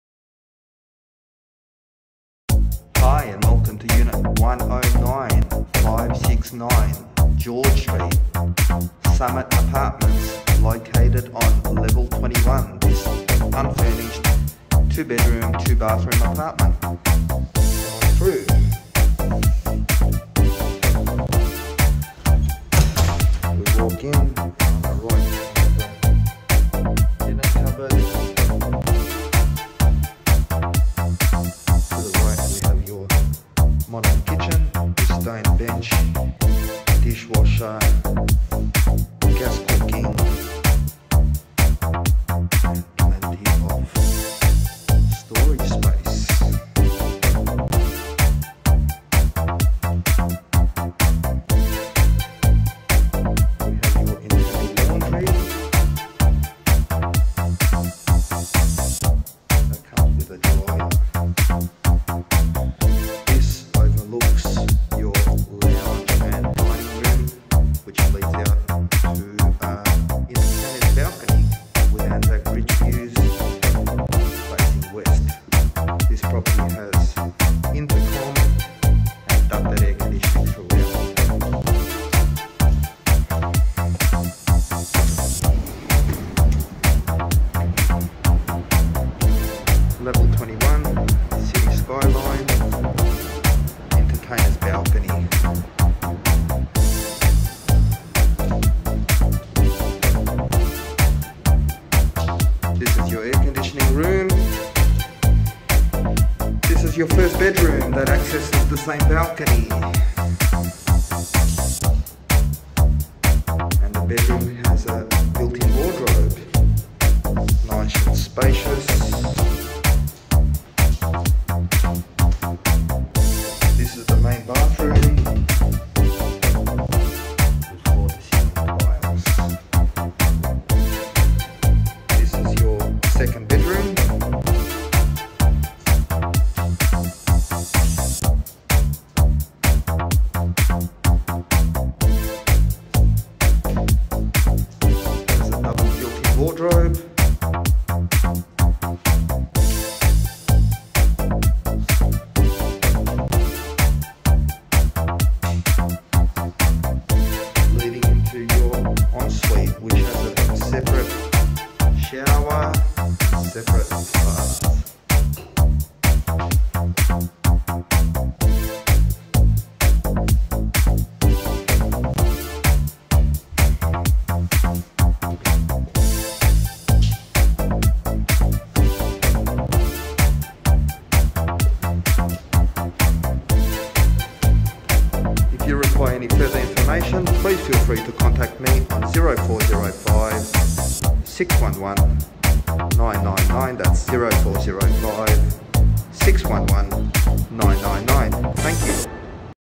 FOR LEASE - 2 Bedroom Unit on Level 21 for rent in heart of Sydney CBD !!!
This unfurnished West facing 2 bedroom apartment is perfectly located in a prime location in the CBD, just 2 minutes walk to Town Hall station, Coles Supermarket, Event Cinema on George Street and 10 minutes walk to Darling Harbour.

With a light-filled living and dining area leading to large balcony, it offers a comfortable, quiet and modern lifestyle.

Property Features:-

- Open plan bright lounge room leading to an entertainers balcony with views
- Timber floors throughout
- 2 Bedrooms with Built In Wardrobes
- Kitchen with gas cooking, dishwasher including stone bench top
- Master bedroom with ensuite and walk-in wardrobe
- 2 Modern Bathrooms ( Main bedroom with Ensuite )
- Internal laundry with dryer
- Building facilities include: Intercom, Lift, Indoor Pool, Spa, Gym, Games Room & BBQ Area

*** Tenant(s) are liable to purchase swipe key for themselves at a cost of $100 per Swipe Key ***

Lease: 6 Months / 12 Months + Extension
NO PETS PERMITTED